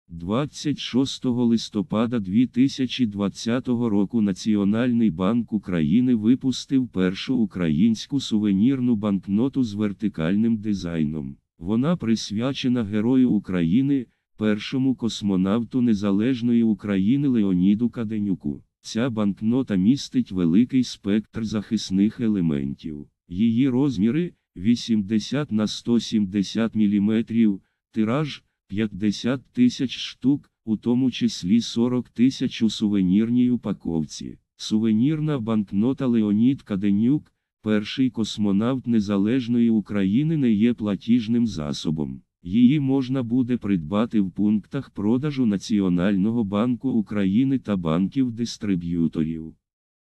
НБУ випустив першу сувенірну банкноту з вертикальним дизайном
26 листопада 2020 року Національний банк України випустив першу українську сувенірну банкноту з вертикальним дизайном. Вона присвячена Герою України, першому космонавту незалежної України Леоніду Каденюку.

Ця банкнота містить великий спектр захисних елементів. ЇЇ розміри - 80 × 170 мм, тираж – 50 000 штук (у тому числі 40 000 у сувенірній упаковці).

Сувенірна банкнота «Леонід Каденюк – перший космонавт незалежної України» не є платіжним засобом. Її можна буде придбати в пунктах продажу Національного банку України та банків-дистриб’юторів.

НБУ випустив першу сувенірну банкноту з вертикальним дизайном. На ній зображений Леонід Каденюк

Нова сувенірна банкнота “Леонід Каденюк – перший космонавт незалежної України”